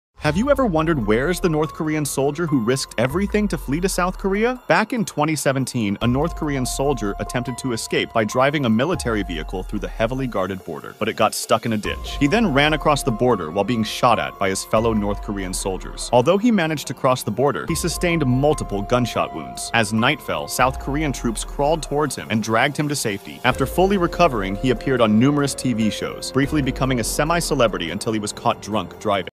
The North Korean Soldier Who Tried To Flee!
Have you ever wondered where is the North Korean soldier who risked everything to flee to South Korea? Back In 2017, a North Korean soldier attempted to escape by driving a military vehicle through the heavily guarded border, but it got stuck in a ditch. He then ran across the border while being shot at by his fellow North Korean soldiers. Although he managed to cross the border, he sustained multiple gunshot wounds. As night fell, South Korean troops crawled towards him and dragged him to safety. After fully recovering he appeared on numerous TV shows, briefly becoming a semi-celebrity until he was caught drunk driving.